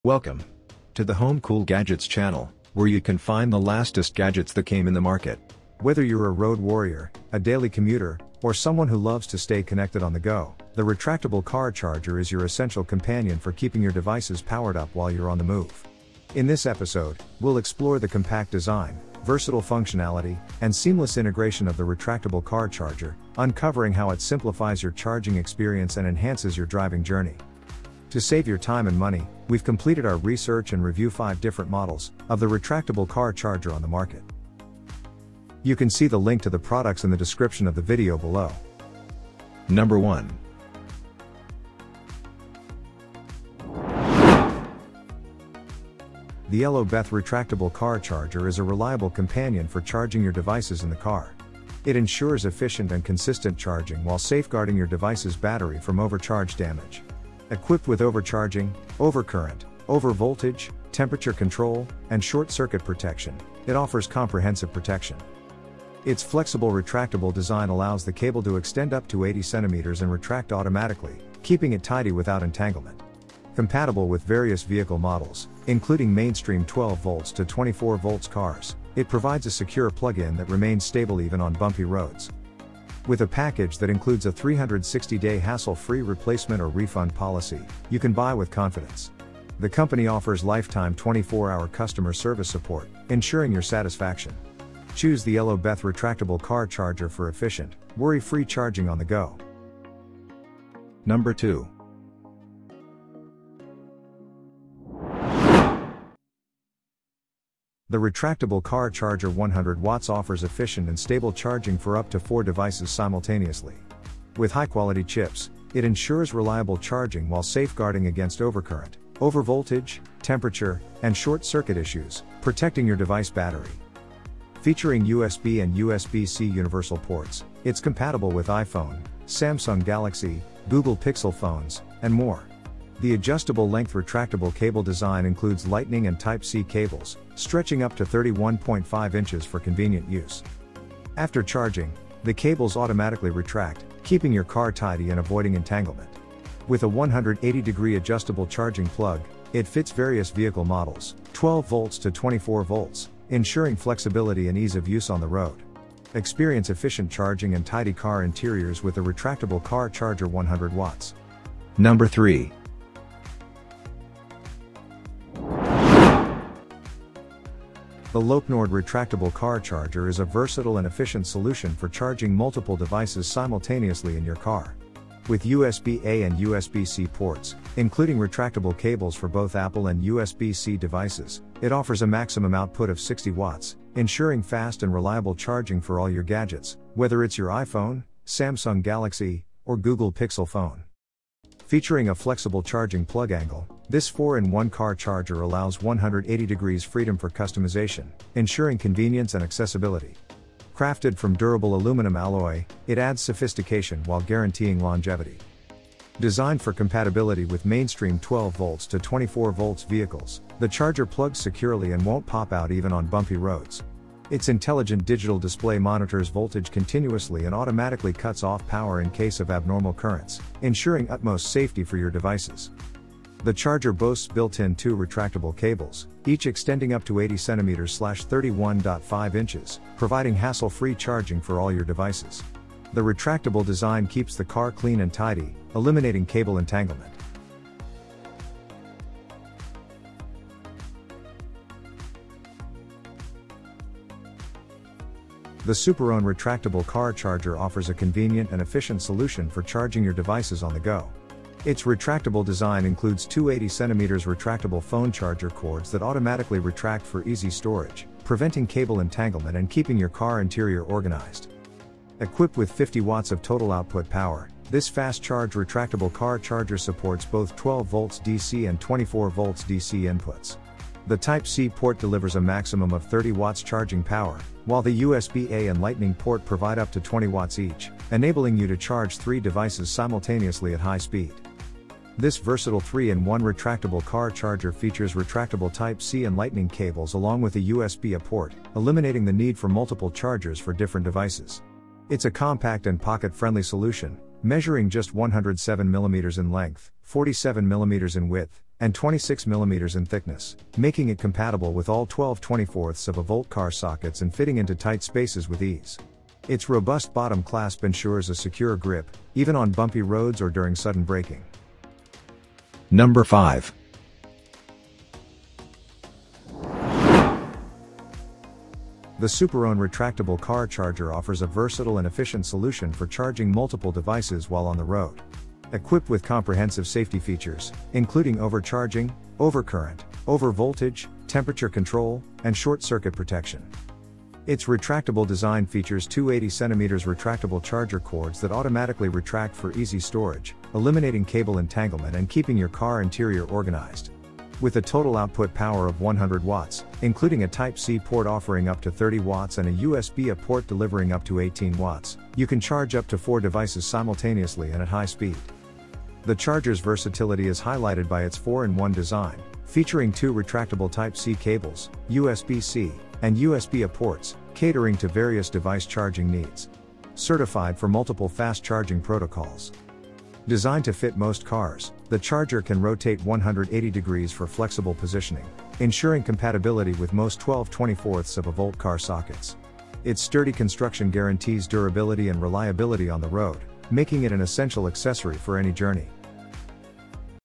Top 5 Best Retractable Car Charger For Iphone And Usb C Amazon 2024
Here Is The Link For Retractable Car Charger 👇👇

Number 1
Number 2
Number 3
Number 4

00:00 Introduction
00:40 EloBeth Retractable Car Charger
01:45 UYOUNG Retractable Car Charger
02:44 Lopnord Retractable Car Charger
04:01 SUPERONE  Retractable Car Charger
05:15 SUPERONE Fast Charge Retractable Car